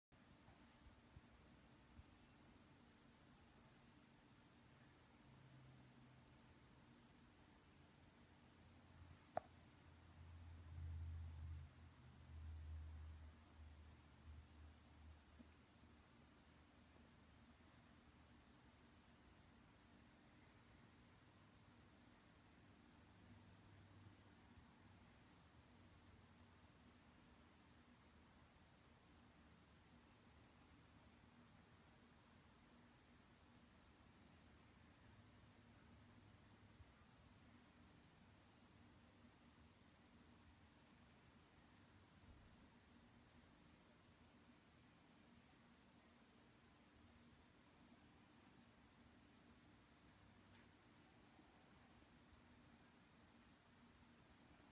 Baby girls first video. Moving in the belly. 39.5 weeks
Finally caught some of her moves on video